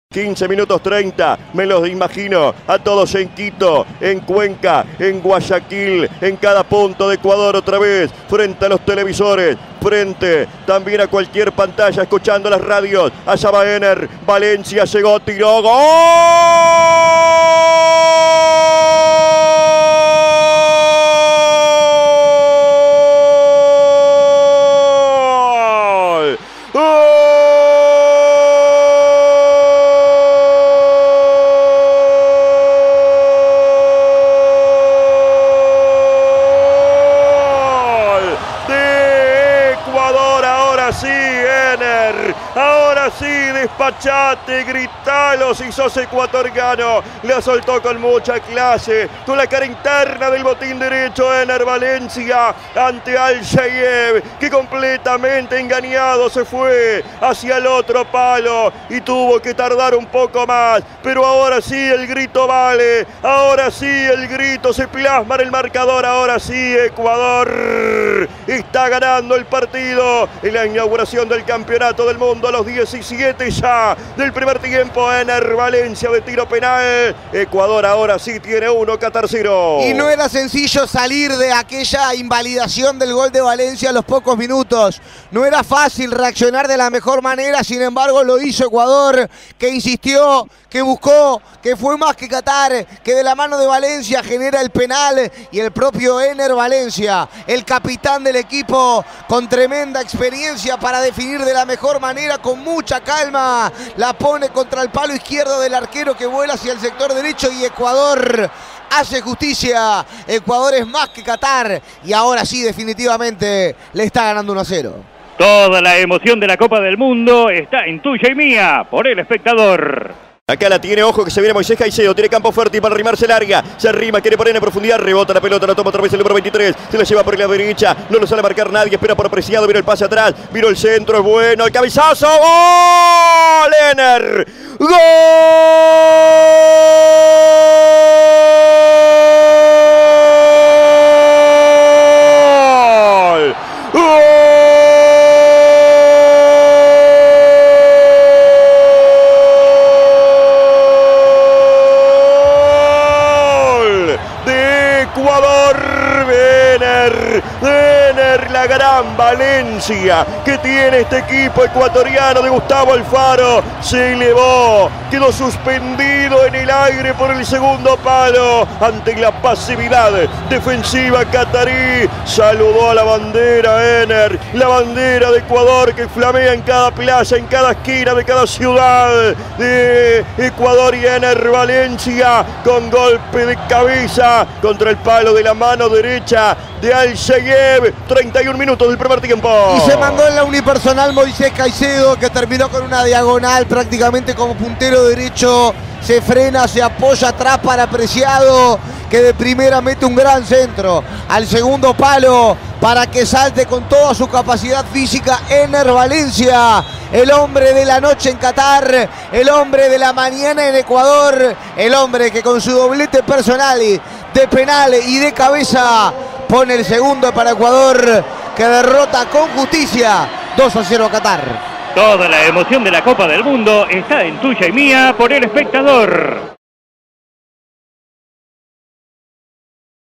Qatar 0 Ecuador 2 - Copa del Mundo 2022 (por Julio Macchi)
Los goles del partido, por Julio Macchi (agradecimiento especial) y Diego Sonsol para Tuya y Mía por El Espectador.